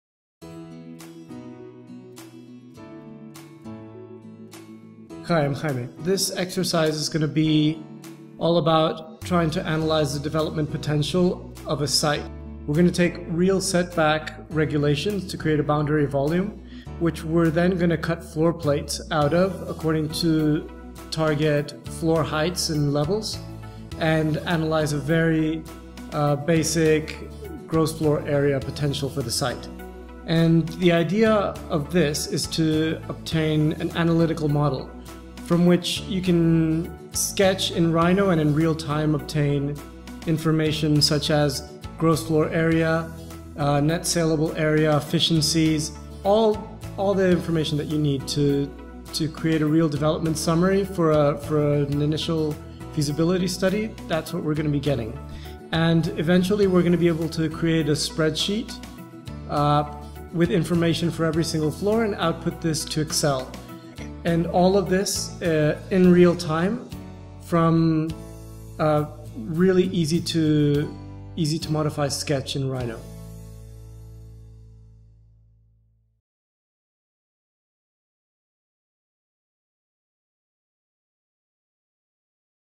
Creating Models for Development Analysis with Grasshopper - Intro
Any good architectural design should always start with a proper assessment of the site´s development potential.

Parametric modeling can assist early on these tasks by allowing the designer to sketch models based on location restrictions quickly. From these early models, we can extract relevant data from the site area, building footprints, site usage, the number of levels, etc. all of which can be used to evaluate the feasibility of the project and inform later design phases.

On this course, Jaime Sol will walk you through the whole process of how you can create an analysis tool with Grasshopper to perform a complete development potential study.